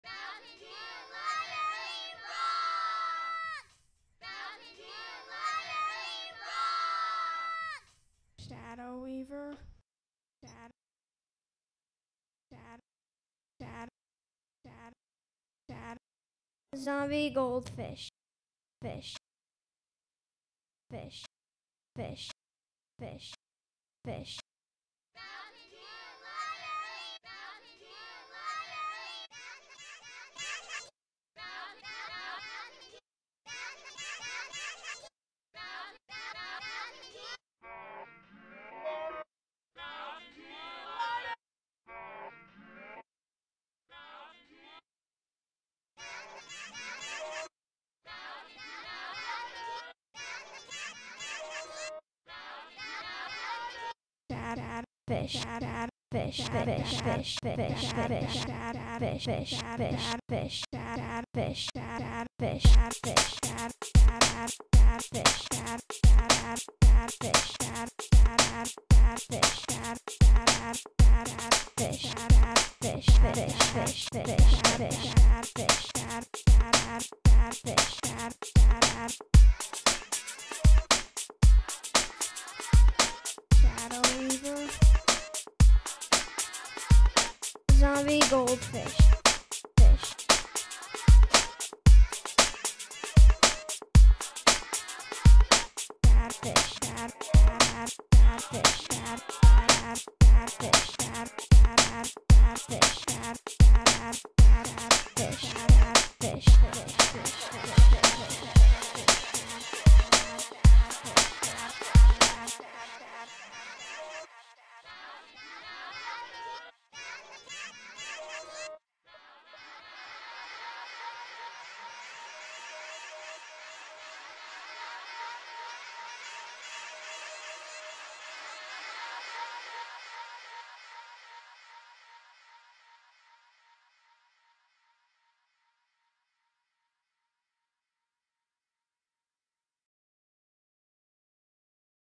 8 14 18 Randle Library
Hear the brand new tune the kids dropped with their original, vocal sounds celebrating how libraries rock and their favorite books they read this summer.